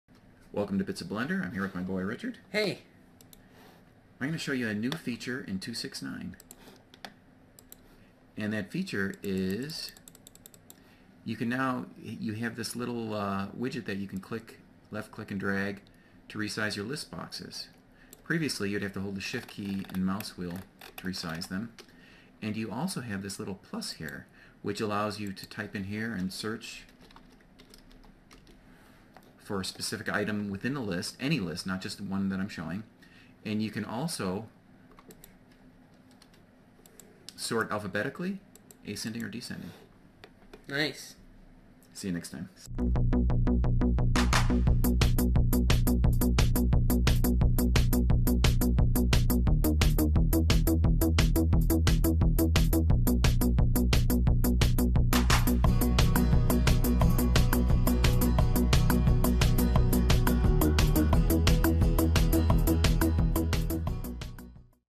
Bits of Blender #63 - Lists in 2.69
Blender 2.69 added some new functionality to the UI with regards to lists. John and Richard Nyquist show the new list features in this Bit.